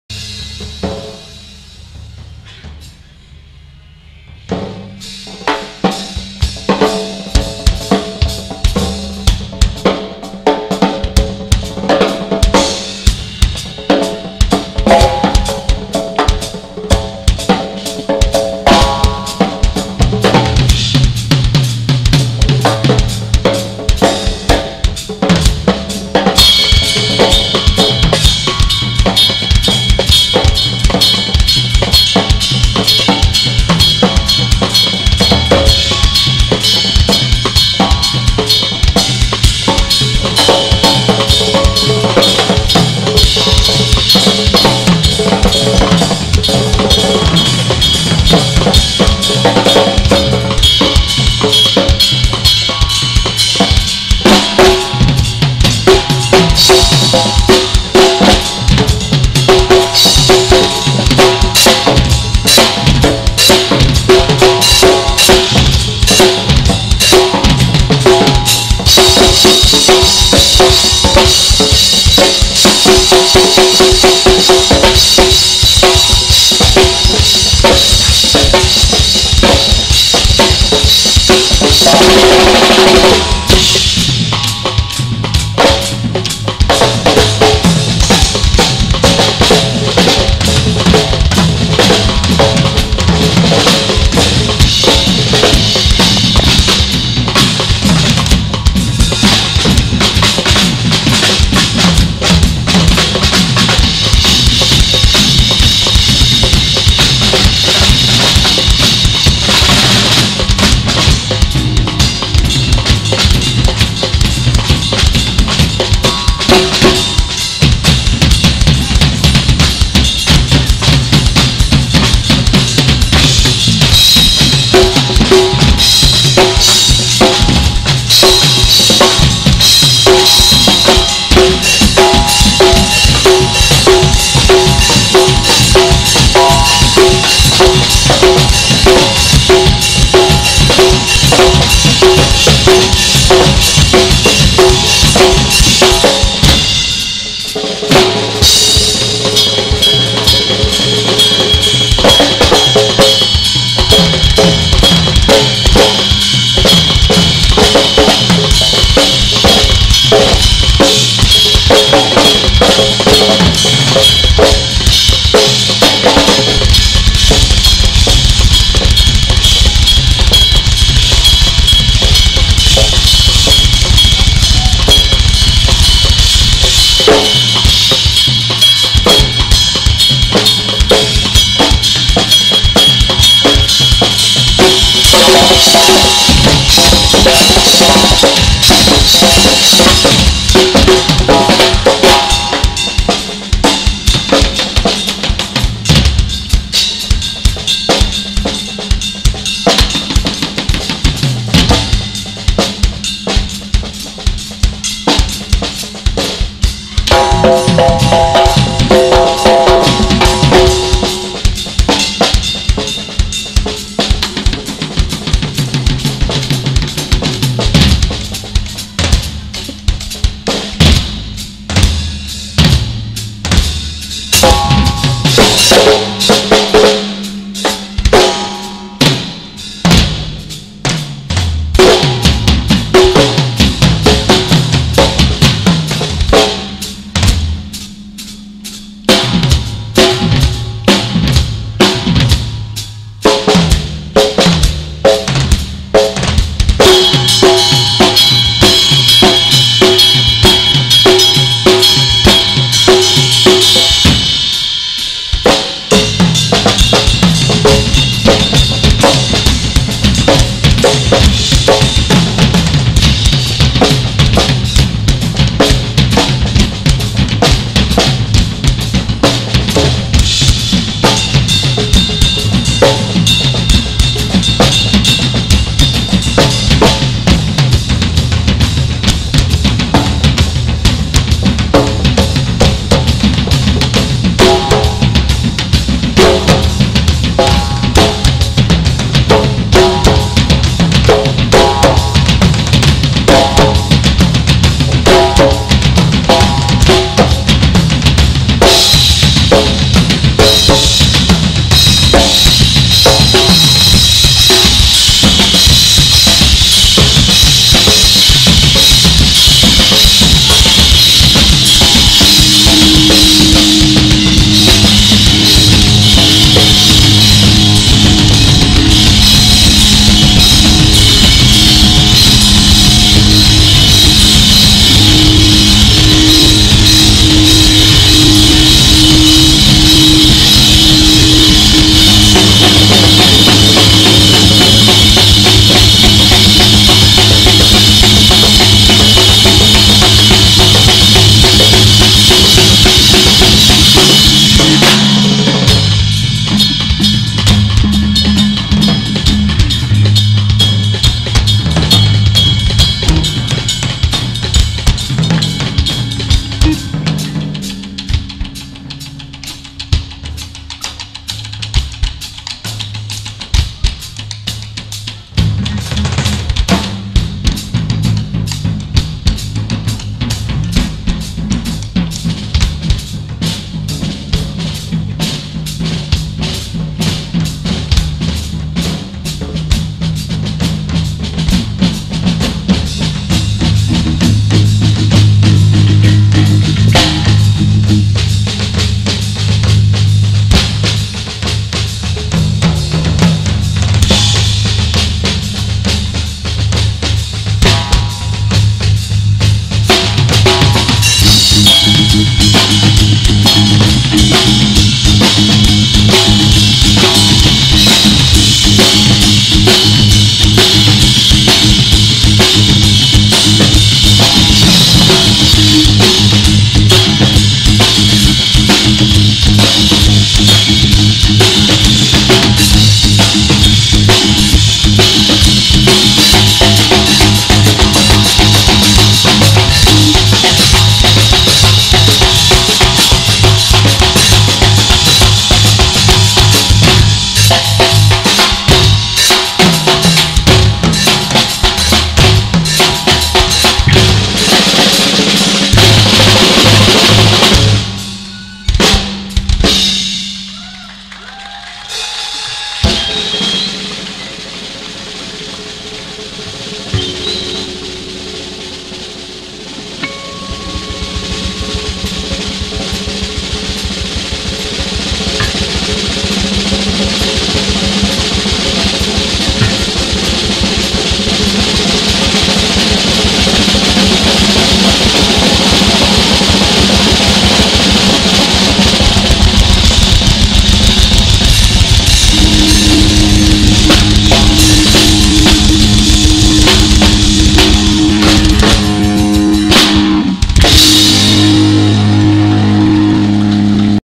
GURU VS MURID - GILANG RAMADHAN VS SEPSA ICHA 3DIOTS
DRUM CONVERSATION GILANG RAMADAHAN VS SEPSA ICHA 3DIOTS
@GATHERING GRSB PEKALONGAN 2015